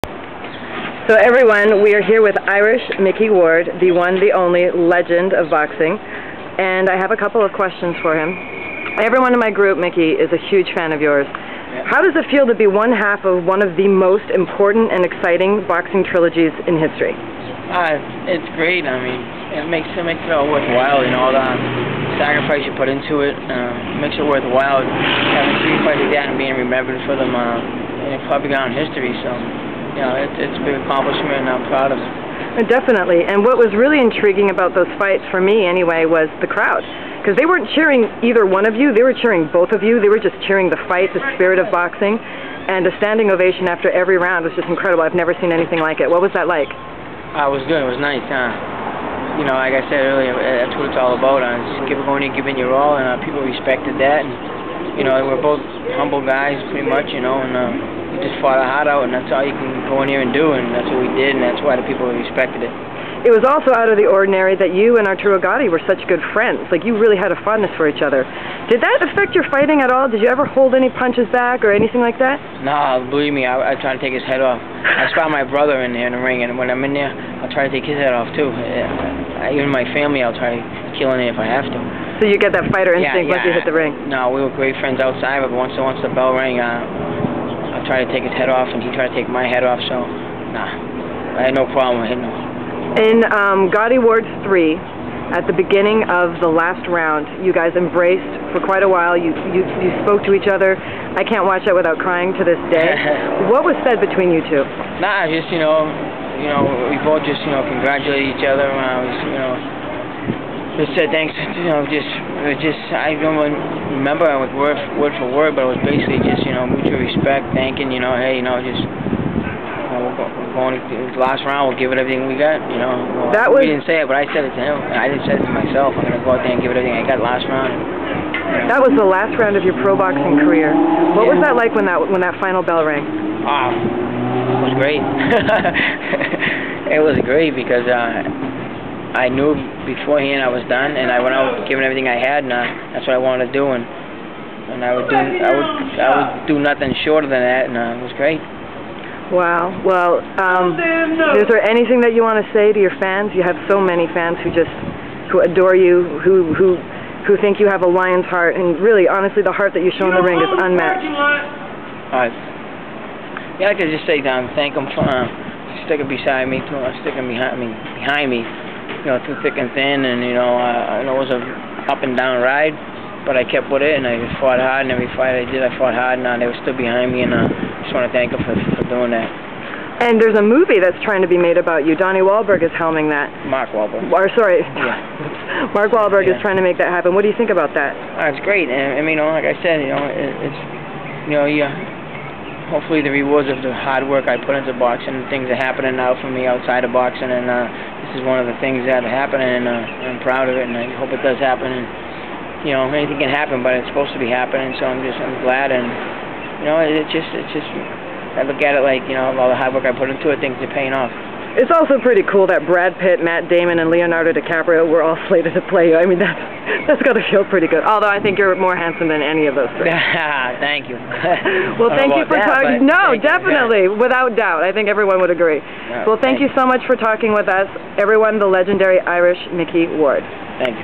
Micky Ward
This interview took place while Irish Micky Ward was in Los Angeles for the filming of the now-released film, The Fighter, which chronicles a slice of Ward's compelling life and relationship with quirky, troubled brother, Dicky Ecklund.

It was also just weeks before Gatti's death :(